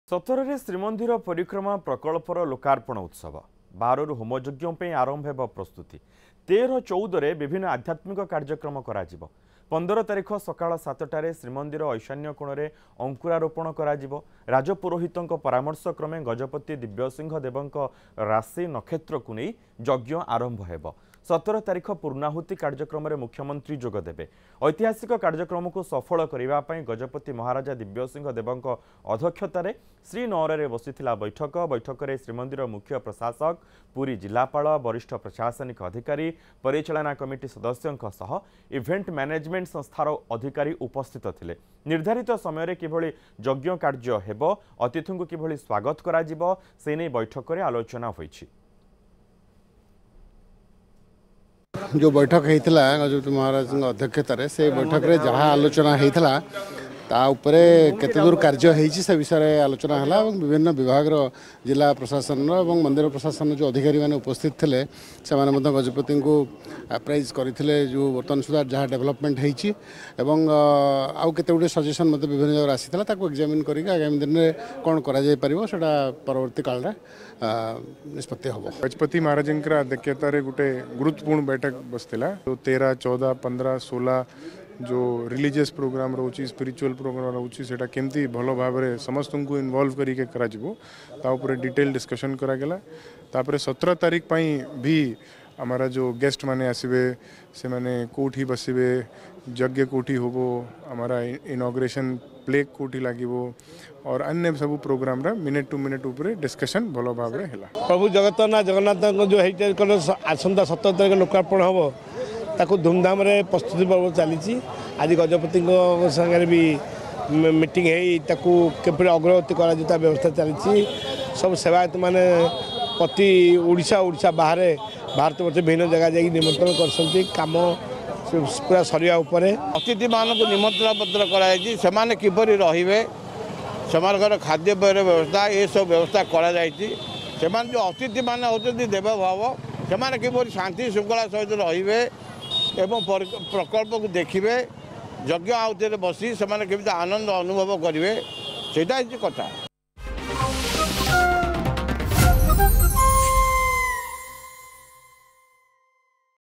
୧୭ ରେ ପରିକ୍ରମା ପ୍ରକଳ୍ପ ଲୋକାର୍ପଣ, ଗଜପତିଙ୍କ ଅଧକ୍ଷତାରେ ସମୀକ୍ଷା ବୈଠକ ଅନୁଷ୍ଠିତ | PrameyaNews7
୧୭ ରେ ପରିକ୍ରମା ପ୍ରକଳ୍ପ ଲୋକାର୍ପଣ, ଗଜପତିଙ୍କ ଅଧକ୍ଷତାରେ ସମୀକ୍ଷା ବୈଠକ ଅନୁଷ୍ଠିତ | Parikrama project meeting at Srinaar | PrameyaNews7